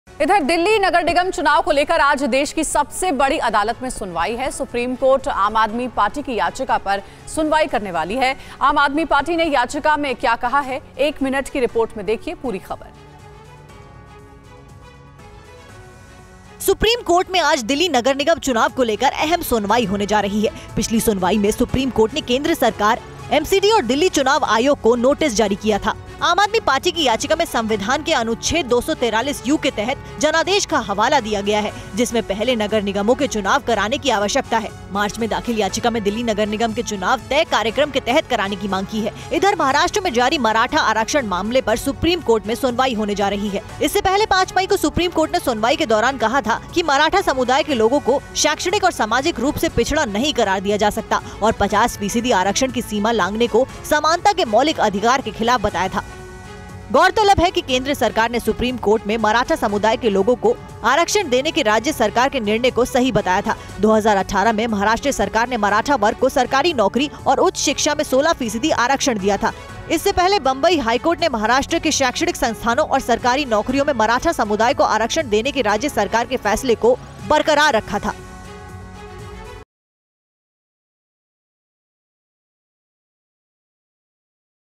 MCD Elections: Delhi नगर निगम चुनाव को लेकर आज देश की सबसे बड़ी अदालत में सुनवाई | SC
सुप्रीम कोर्ट में आज दिल्ली नगर निगम चुनाव को लेकर अहम सुनवाई होने जा रही है... पिछली सुनवाई में सुप्रीम कोर्ट ने केंद्र सरकार, एमसीडी और दिल्ली चुनाव आयोग को नोटिस जारी किया था.